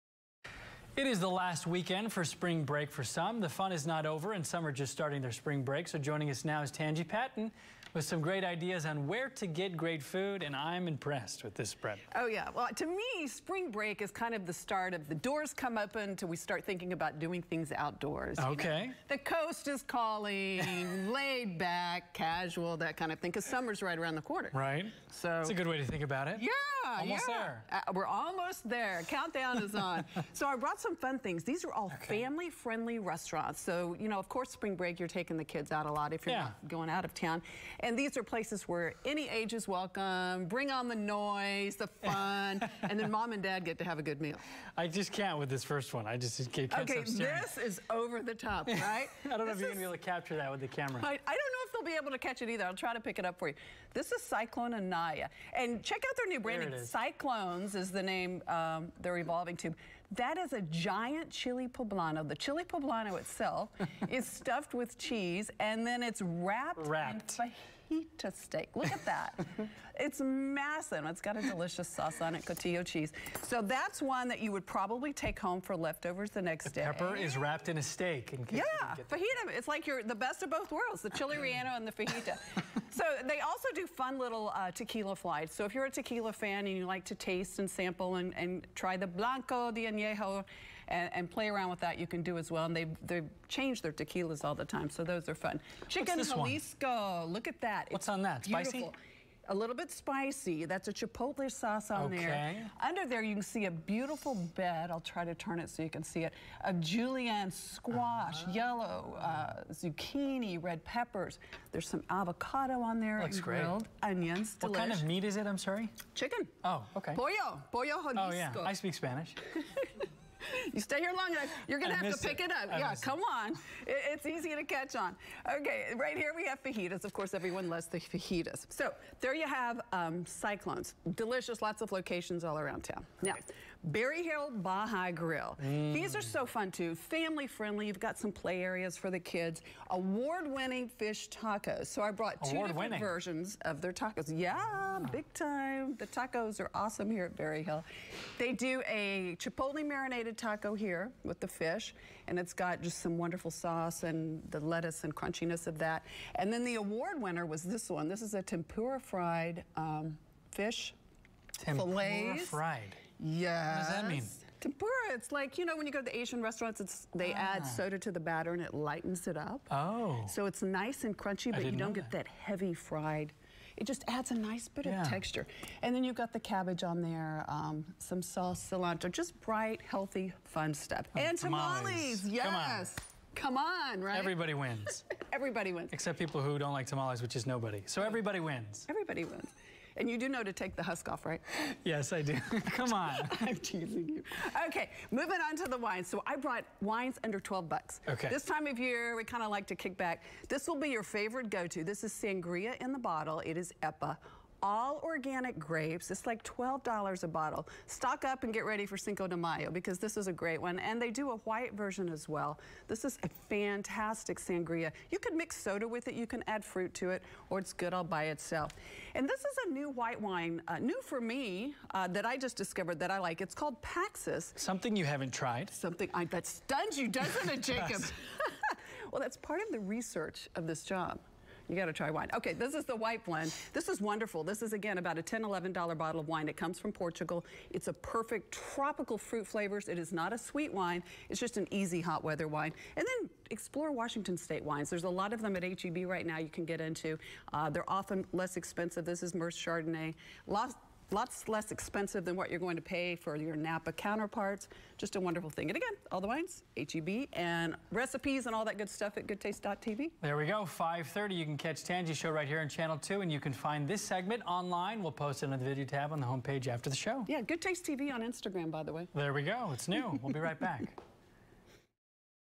Try Out These Restaurants Before Spring Break Ends
Tanji Patton shares her top picks on where to get great food this Spring Break